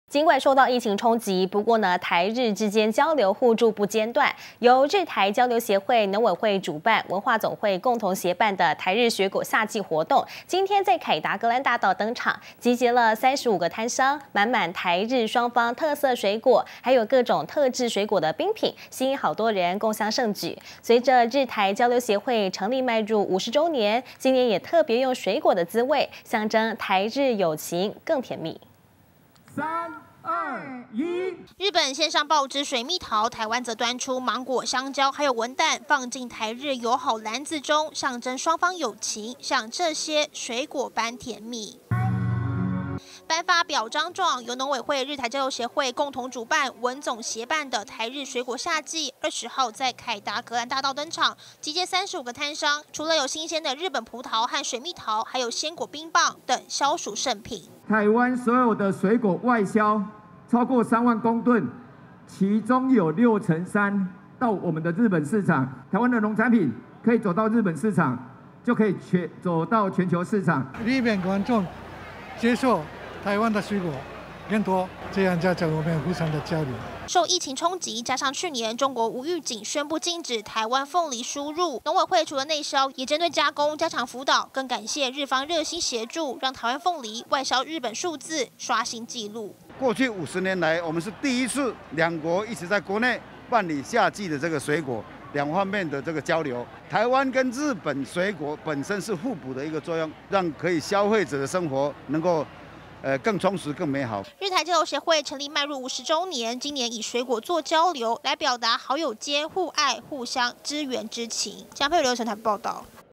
「台日水果夏祭」凱道登場 集結特色水果、冰品
儘管受到疫情衝擊，但台日之間交流互助不間斷。由日台交流協會、農委會主辦，文化總會共同協辦的「台日水果夏祭」活動，今天（２０號）在凱達格蘭大道登場，集結35個攤商，滿滿台日雙方的特色水果、還有各種特製水果冰品，吸引大批民眾共襄盛舉。隨著日台交流協會成立邁入50週年，今年特別用水果的滋味，象徵台日友情更甜蜜。